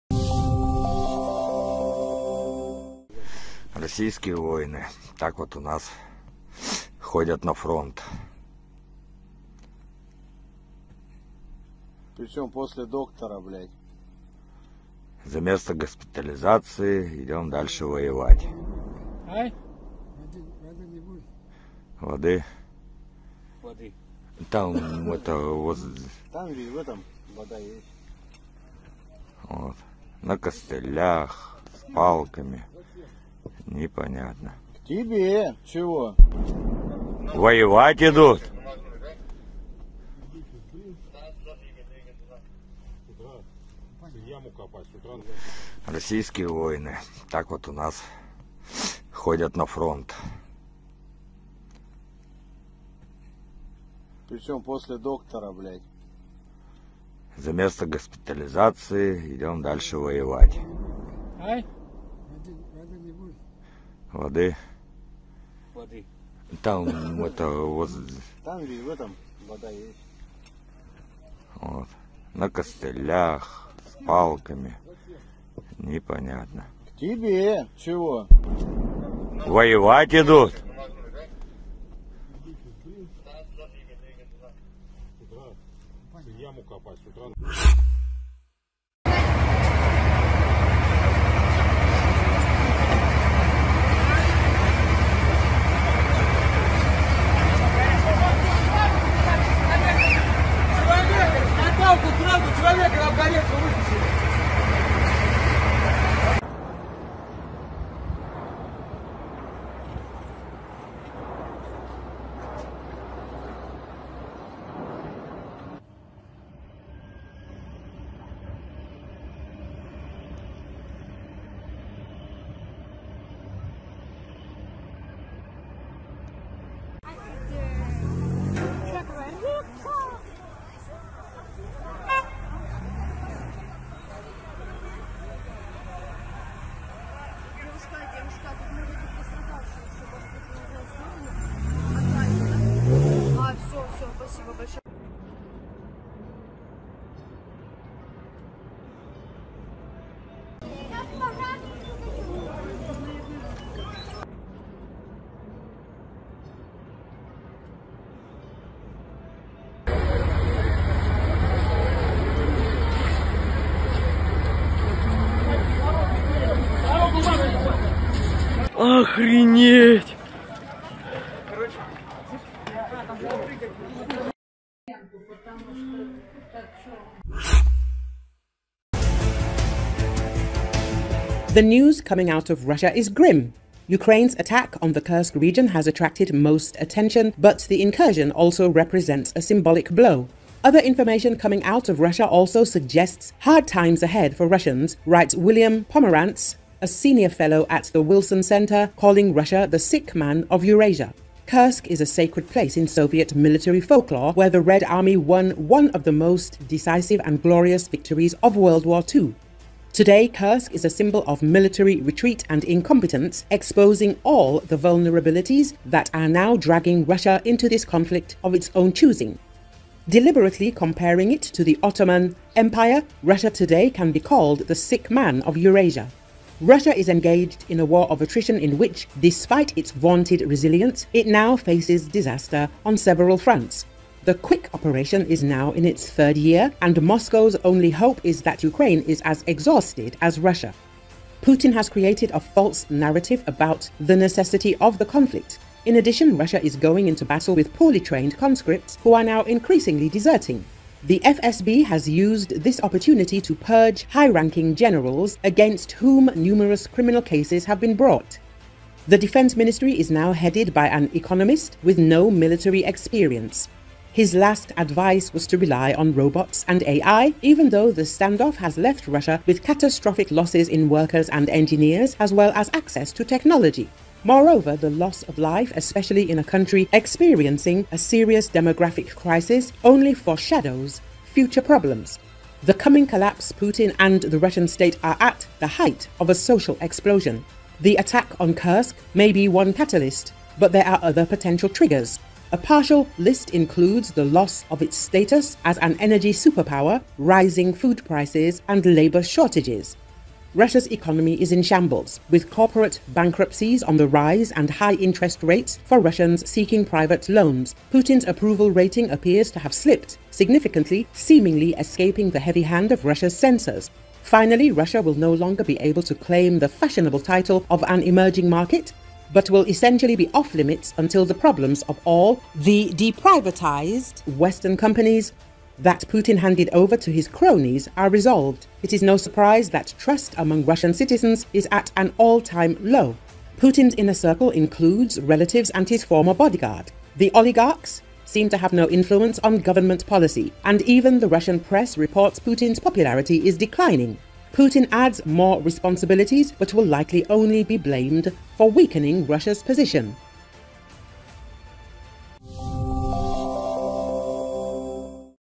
Footage from frontline of wounded Russian soldiers sent into battle with crutches
A video of wounded soldiers of the occupying Russian army from the front line has been released. As can be seen from the footage taken and shared by the Russian military, two soldiers of the occupying army who moved with crutches were injured and sent back to the front without full recovery.